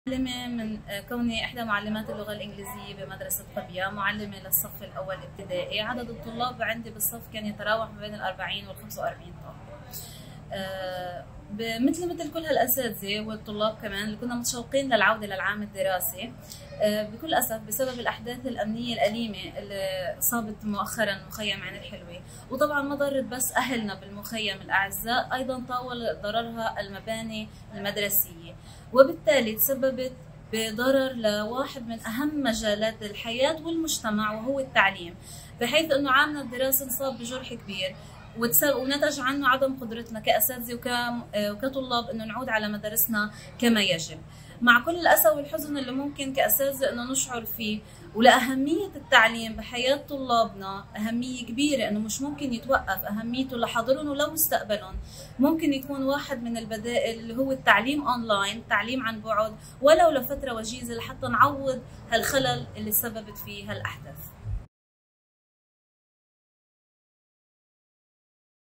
World Teacher's Day 2023 - Voices from Ein El Hilweh, Lebanon
Rachelle Saadi is an UNRWA teacher in Ein El Hilweh camp, Lebanon. Watch as she shares her experience teaching Palestine Refugee students.